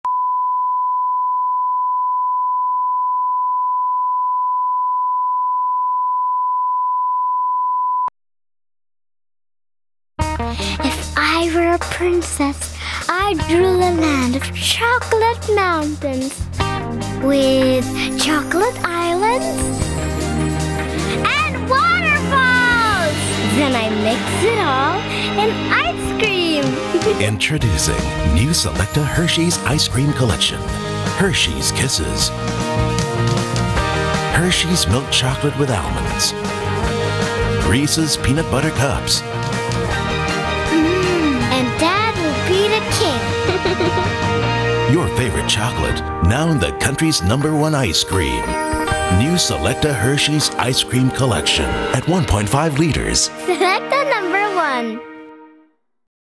Selecta Hershey's Ice Cream Collection 45 Seconds Commercial Video
It's the time of the year again where children are taking their off from school for summer vacation and they are either heading to beach or sports clinic. But aside from recreational activities this summer, one of the best thing that parents should do is to bond with their family even at home. This is where Selecta Ice Cream comes in the picture from the "masarap kasama sa weekend" it moves to "masarap ka-bonding this summer" with their latest treat to all -- the Selecta Hershey's Ice Cream Collection.

The new Selecta Hershey's Ice Cream Collection comes in four different flavors to bring out the pure happiness in every one. It includes Hershey's Milk Chocolate, Milk Chocolate with Almonds, Reese's Peanut Butter Cups, and Kisses flavors.